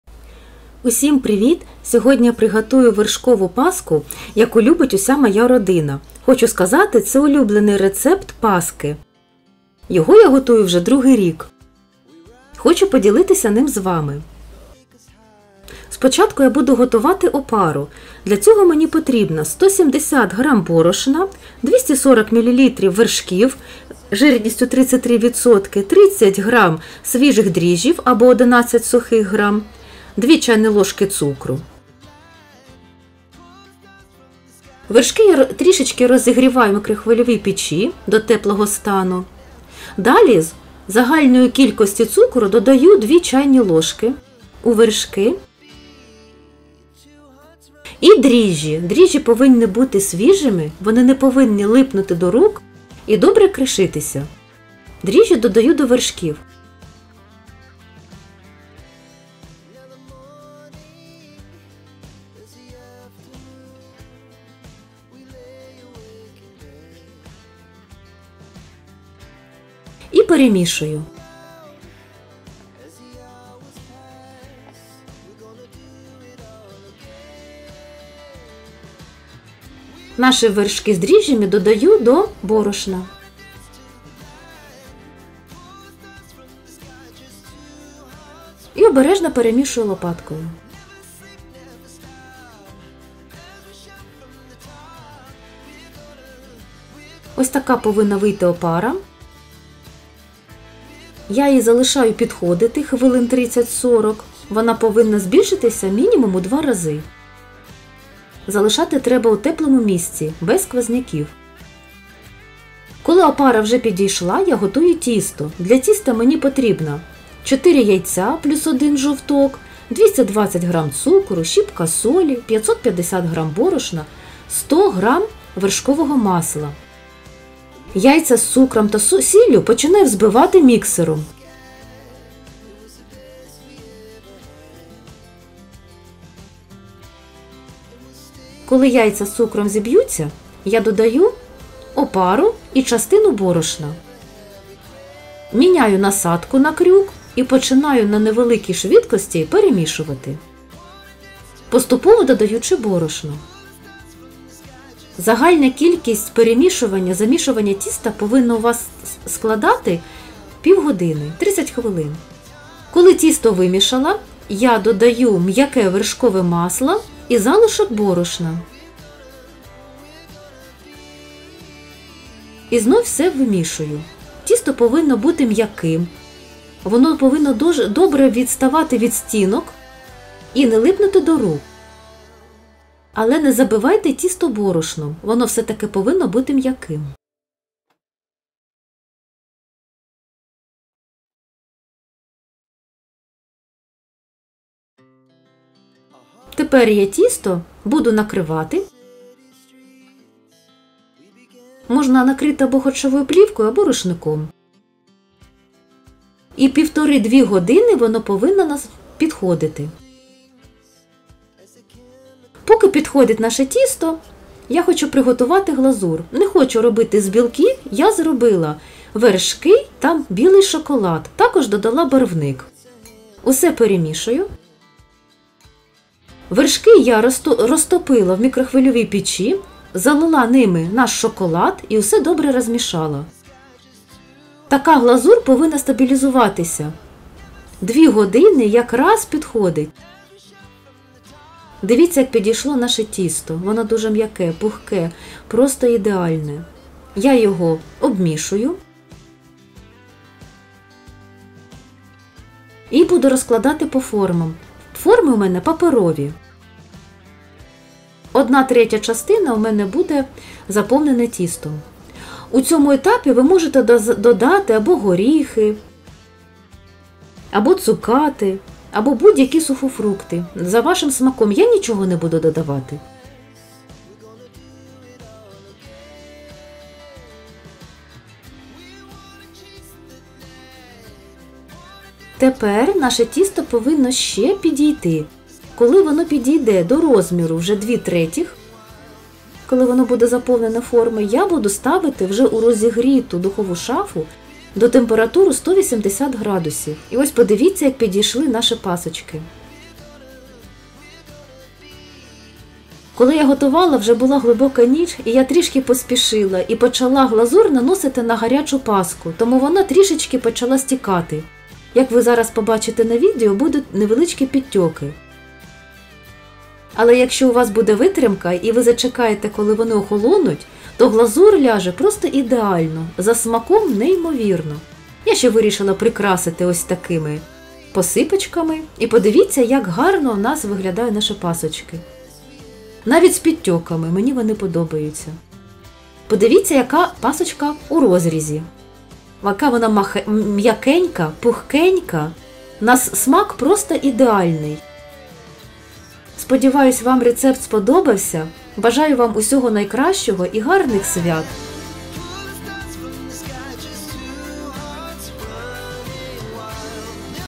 ВЕРШКОВА ПАСКА - САМА НІЖНІСТЬ!! + ШОКОЛАДНА ГЛАЗУР
ВЕРШКОВА ПАСКА - САМА НІЖНІСТЬ!!

Рецепт:
Опара:
170 гр - борошна
240 мл - теплих вершків від 33%
30 гр - свіжих дріжджів або 11 гр сухих
2 ч.л. - цукру з загальної кількості

Тісто:
уся опара
4 яйця+1 жовток
220 гр - цукру
щіпка солі
550 гр - борошна
100 гр - м'якого вершкового масла
за бажанням + 150 гр - цукат, горіхи, сухофрукти, родзинки
Випікати при температурі 180С до сухої шпажки

Глазур:
45 гр - вершків 33% або молока
90 гр - шоколаду
1ч.л. - вершкового масла
барвник - якщо будете використовувати білий шоколад,

СМАЧНОГО!!!

CREAM PASKA - THE VERY GENTLENESS!!

Recipe:
Sponge:
170 g - flour
240 ml - warm cream from 33%
30 g of fresh yeast or 11 g of dry yeast
2 tsp - sugar from the total amount

Dough:
all the stuff
4 eggs + 1 yolk
220 g of sugar
a pinch of salt
550 g - flour
100 g - soft butter
if desired + 150 g - candied fruit, nuts, dried fruits, raisins
Bake at a temperature of 180C until a dry skewer

Glaze:
45 g - 33% cream or milk
90 g of chocolate
1 tsp - butter
dye - if you use white chocolate,

BON APPETIT!!!

ТОРТ НІЖНИЙ ПОЦІЛУНОК

ЛАТТЕ, КАПУЧИНО

ТОРТ НІЖНІСТЬ

ТОРТ ПІНЬЯТА

ЙОГУРТОВИЙ КРЕМ ДЛЯ ТОРТА

САЛАТ ВЕНЕЦІЯ